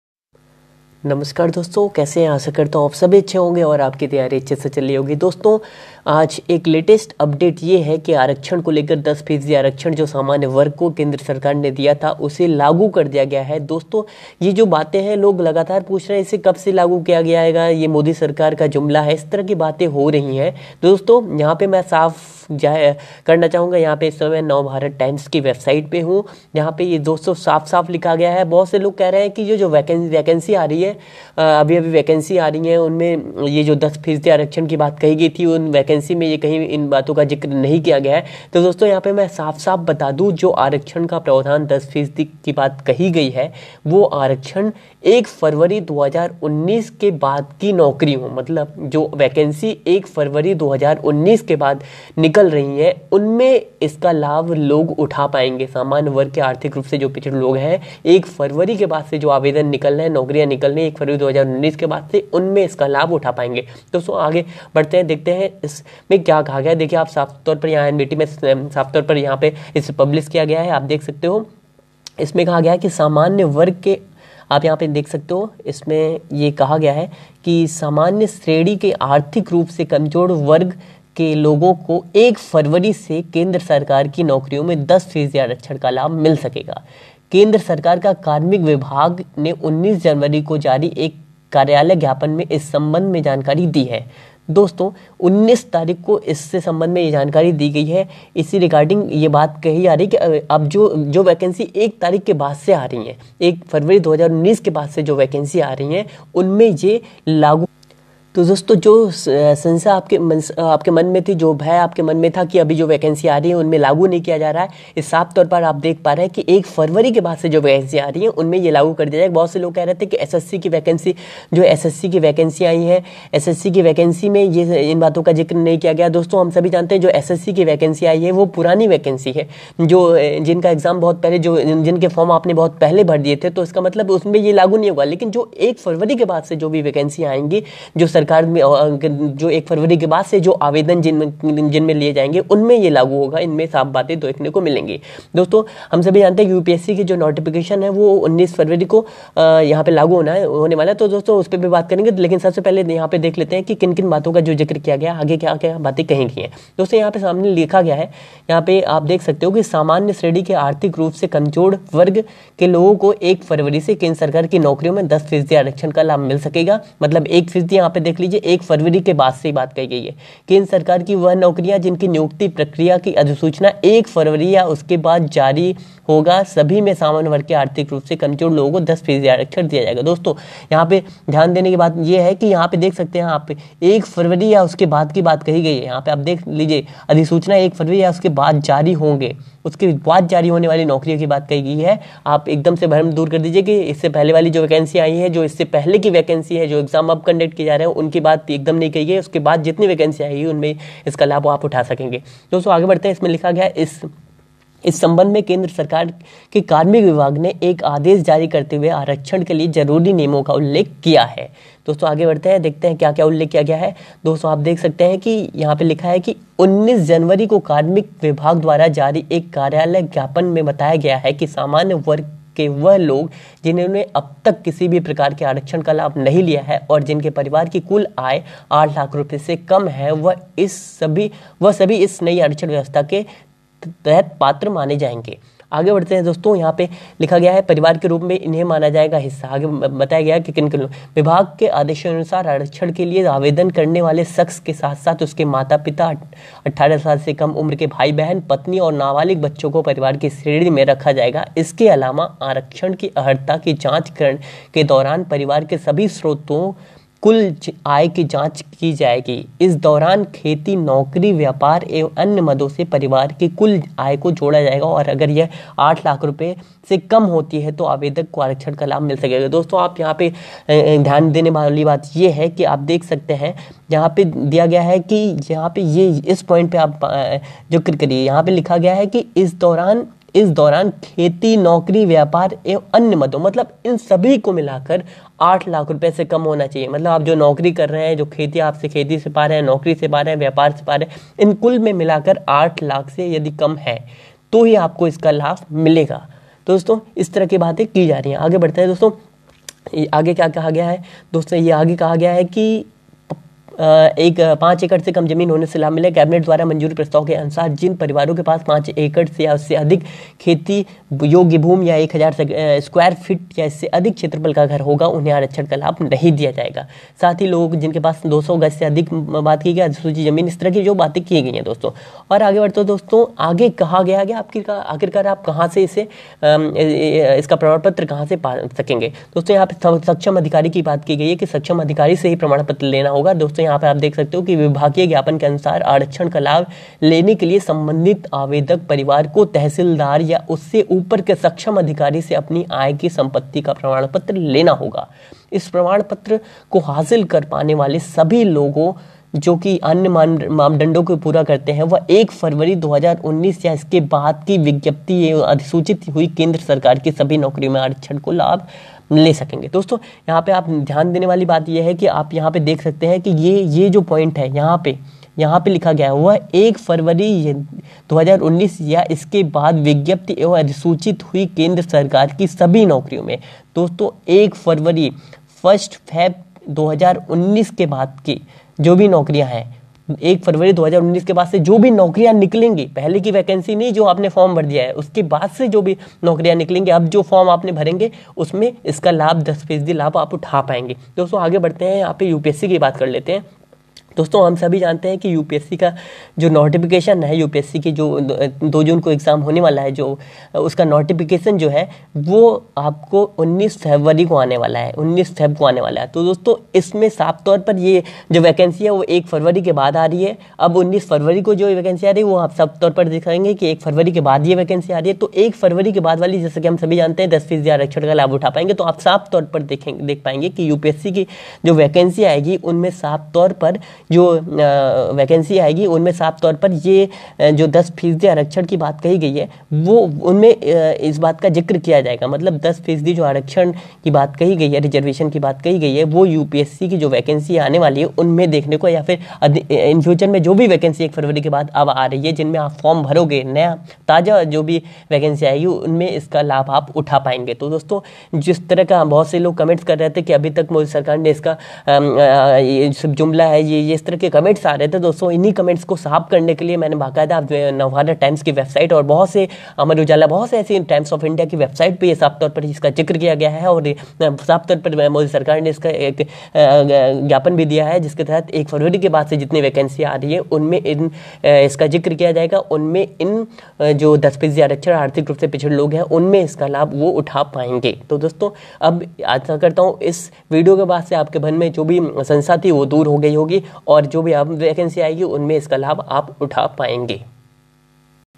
1 फरवरी 2019 से आने वाली Vacancies, में 10% आरक्षण लागू... Govt. implemented 10% Reservation in UPSC
The Government of India has decided to implement 10% Reservation for Economically weak Category from 1st February 2019.